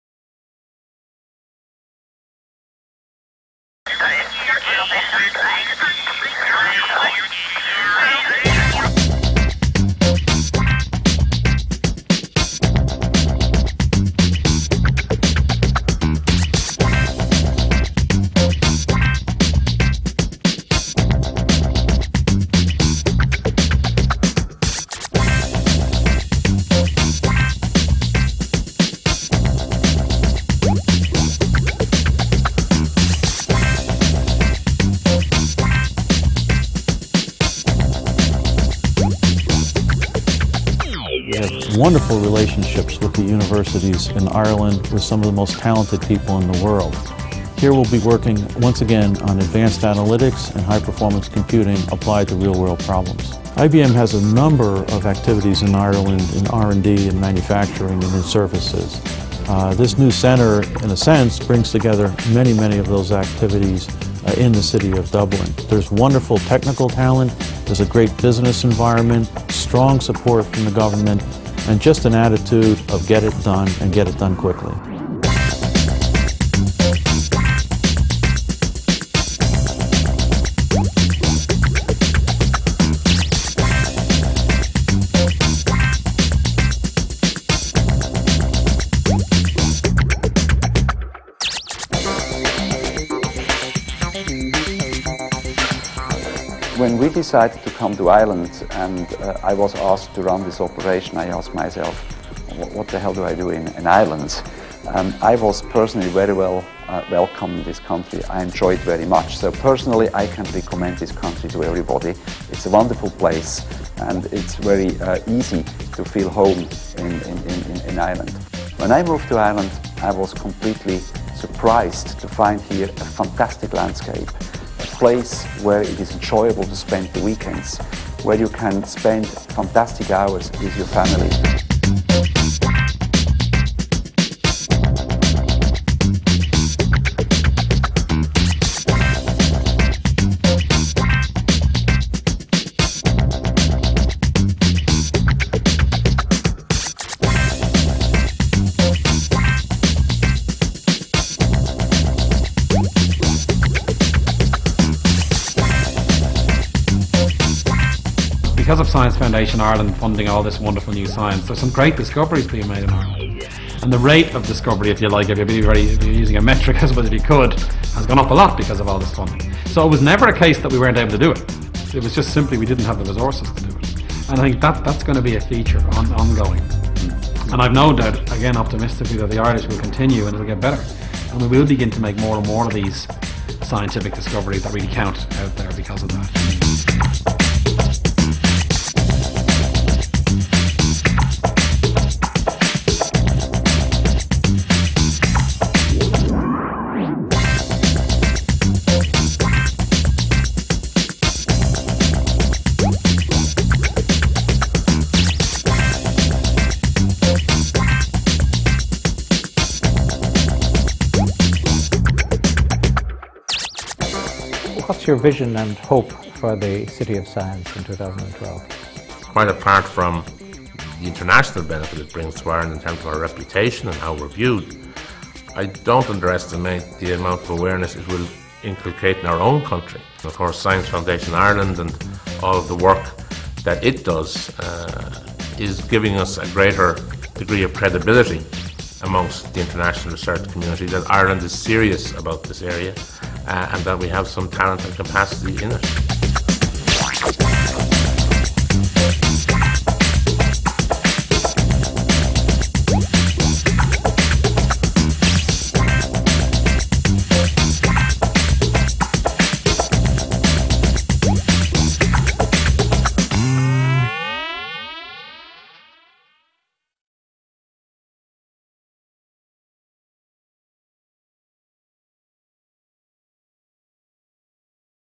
Dublin City of Science 2012
Dublin will host the Euroscience Open Forum from 11-15 July 2012. This inter-disciplinary meeting will bring together 6,000 scientists, business leaders and international media to discuss the best of European science, research and innovation. So, come to Dublin for an intellectually, culturally and socially invigorating experience.